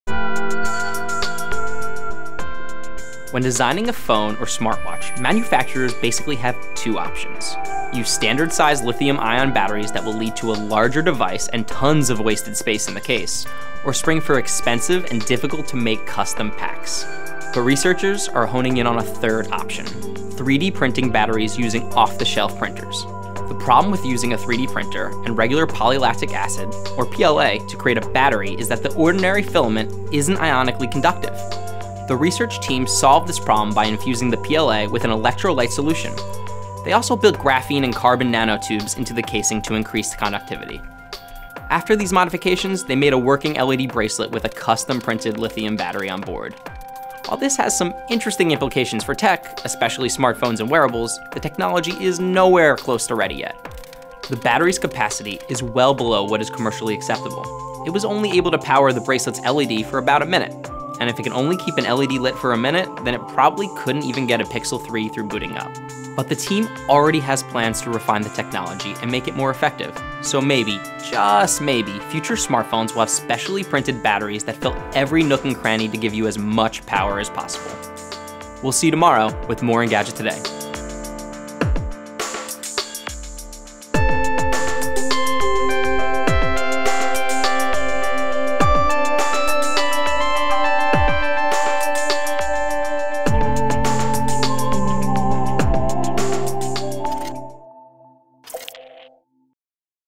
A 3D printed li-ion battery has been created | Engadget Today
One of the challenges in creating smaller and smaller devices these days, such as wearables and phones, is that the batteries can take up a lot of room. Cases are often designed around standard battery sizes, and it often creates wasted space. Now, new research published in ACS Applied Energy Materials shows that it's possible to 3D-print lithium-ion batteries into whatever shape you need.